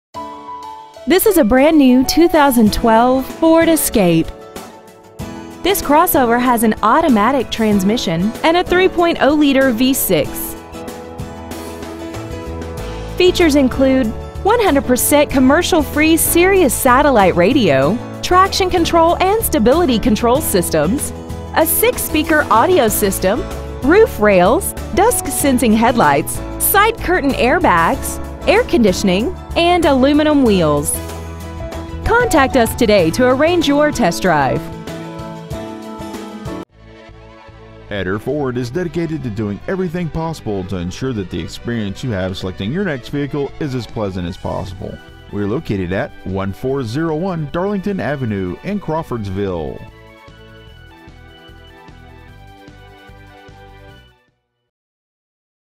2012 Ford Escape Crawfordsville IN
We have been honored to serve the Crawfordsville IN area , we promise that your experience at our dealership will exceed your expectations !

 Year : 2012
 Make : Ford
 Model : Escape
 Engine : 3.0L V6 24V MPFI DOHC FLEXIBLE FUEL
 Trans . : 6-SPEED AUTOMATIC
 Exterior : WHITE
 Miles : 5

 Stock : T642